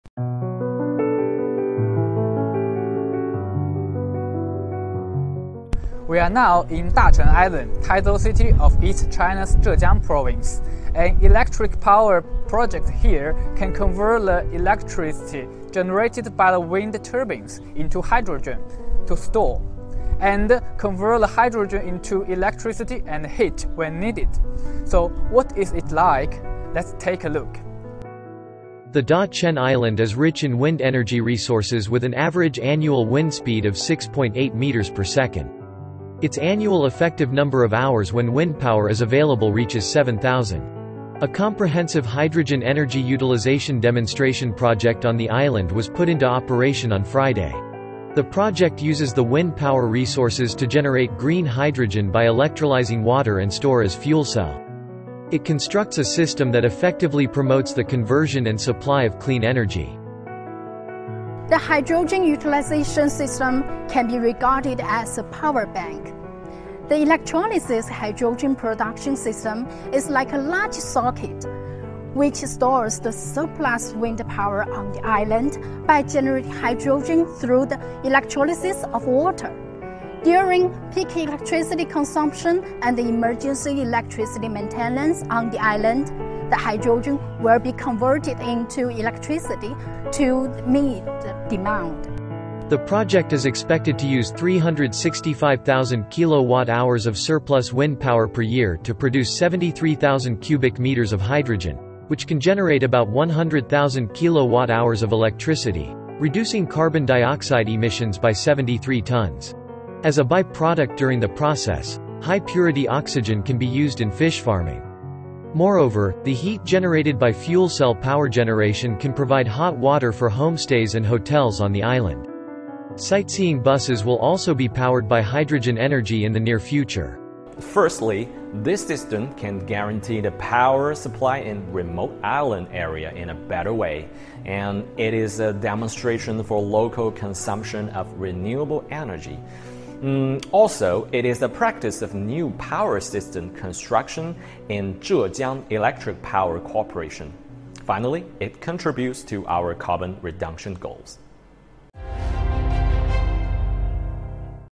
GLOBALink | Green hydrogen project in E China contributes to low-carbon development
A comprehensive hydrogen energy utilization demonstration project on east China's Dachen Island was put into operation. It utilizes wind power resources to generate green hydrogen, promoting conversion and supply of clean energy.